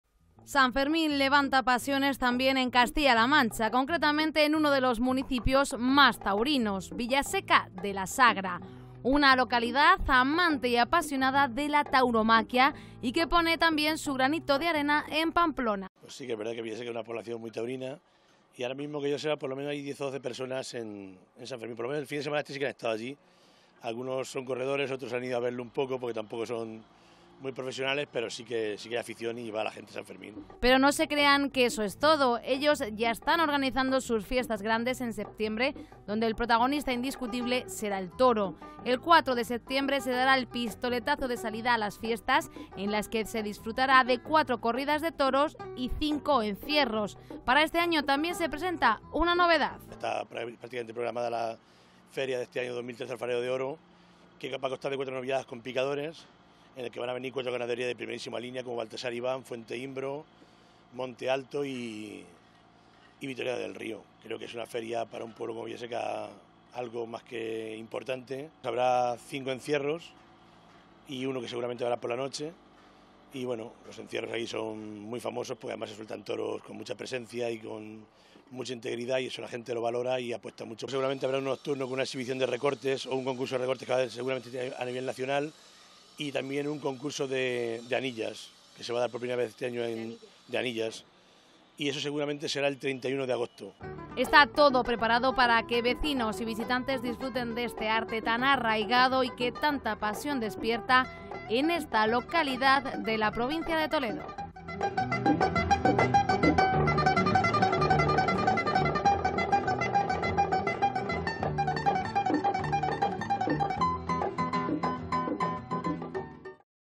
Villaseca de la Sagra se suma a las Fiestas de San Fermín.
Segundo día de encierros  en Pamplona  y muchos Castellanomanchegos se suman a esta tradicional Fiesta de "Los San Fermines".

Hemos visitado una de las localidades con más arraigo al mundo del toro, Villaseca de la Sagra para conocer como se viven estos días y los preparativos para sus fiestas en septiembre.
San Fermín, levanta pasiones también en Castilla La Mancha concretamente; en uno de los Municipios más taurinos; Villaseca de la Sagra, una localidad amante y apasionada de la tauromaquia...Y que pone su granito de arena en Pamplona.

Pero no se crean que eso es todo; ellos ya están organizando sus fiestas grandes en septiembre donde el protagonista indiscutible será el toro. El 4 de septiembre se dará el pistoletazo de salida a las Fiestas en las que se disfrutarán de 4 corridas de toros y cinco encierros. Para este año también se presenta una novedad.

Está todo preparado para que vecinos y visitantes disfruten de este arte tan arraigado y que tanta pasión despierta en esta localidad de la provincia de Toledo.